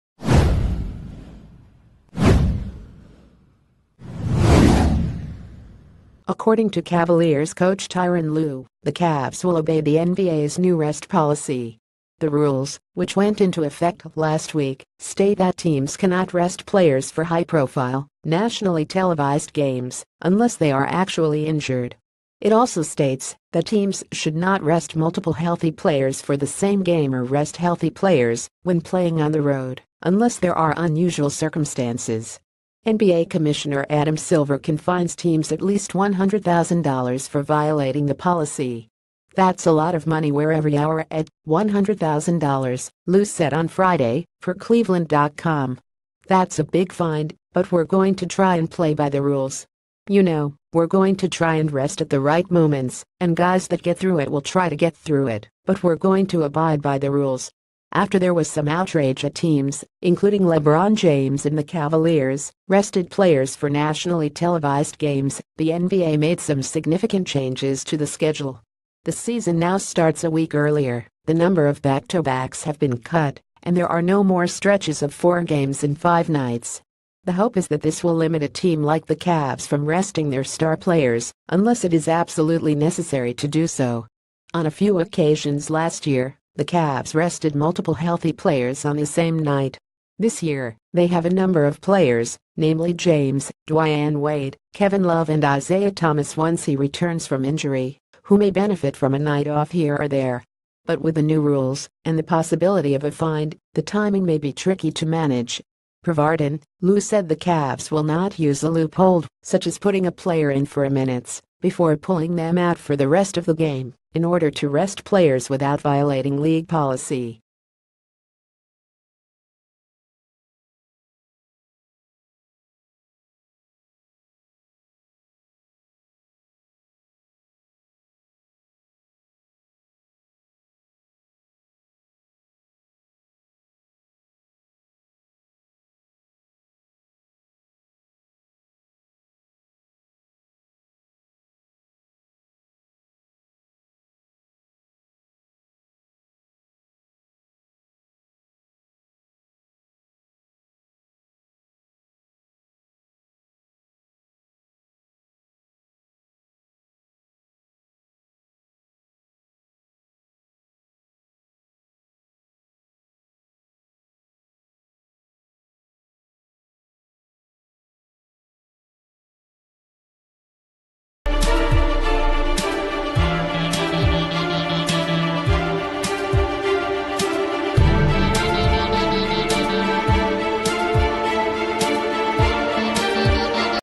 Breaking News | Tyronn lue says the cavs will follow the nba’s new rest policy
Tyronn lue says the cavs will follow the nba’s new rest policy

According to Cavaliers coach Tyronn Lue, the Cavs will obey the NBA’s new rest policy. The rules, wh...